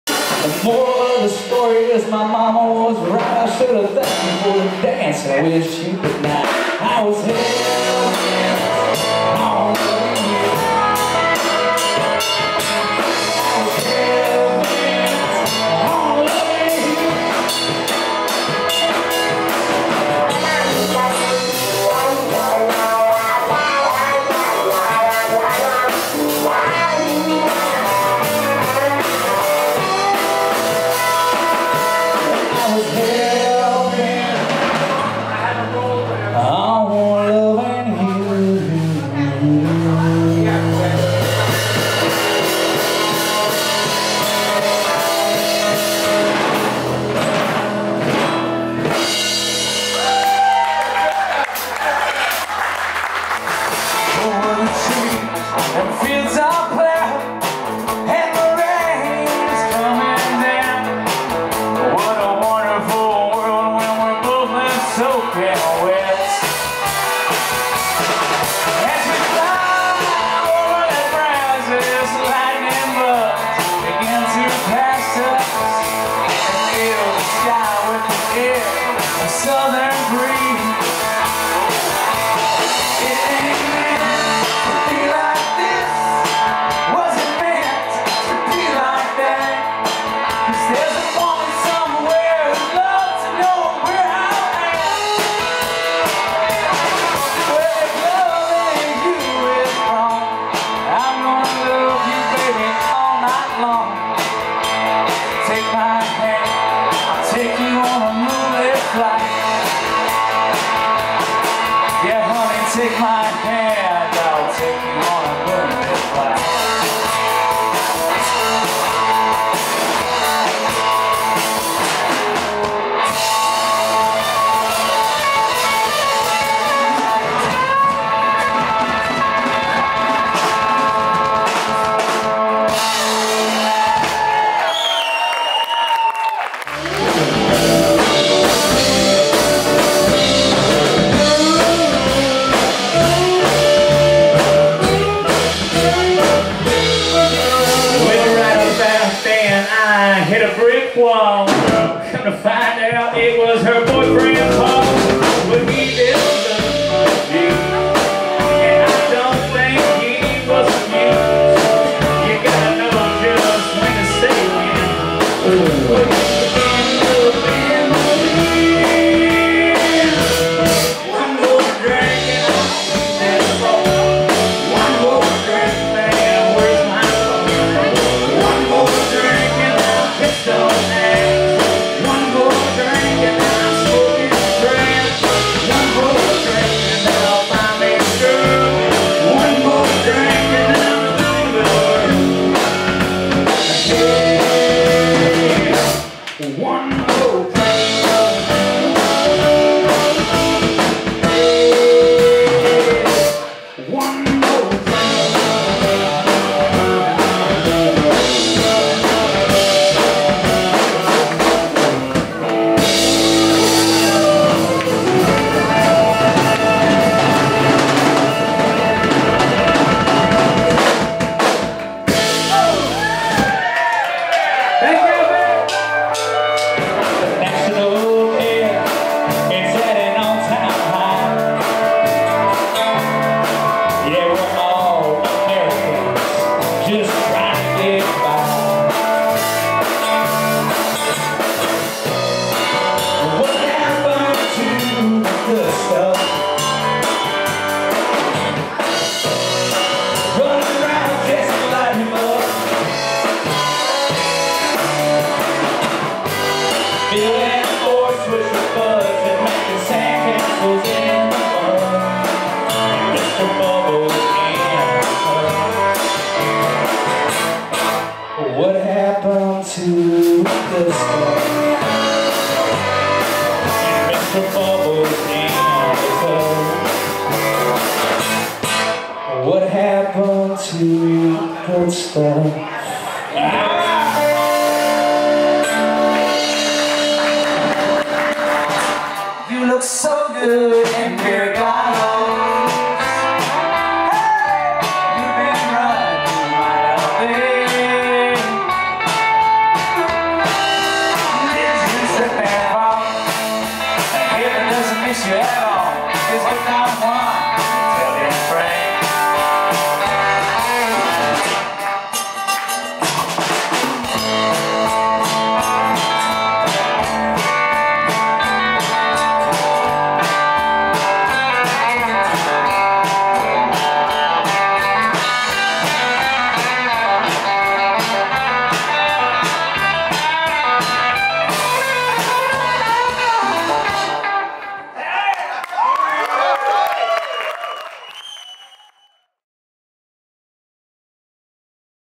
Cody Joe Hodges @ Montauk Music Fest, New York - part 1
Cody Joe and the boys did a might fine job at the Montauk Music Fest May 15-17th, 2015.

Cody Joe Hodges on the Breedlove Nylon String Guitar

 featuring:
Andy Garsea - Electric STAGG Upright Bass Guitar
Willie Velasquez - Electric Guitar
Santi Tani - Backlined Drum Kit

Venues featured -
The Sloppy Tuna, The "Green" Pavilion, ZUM Schneider

Venues not featured:
Montauket, The Point, Swallow East, East by North-East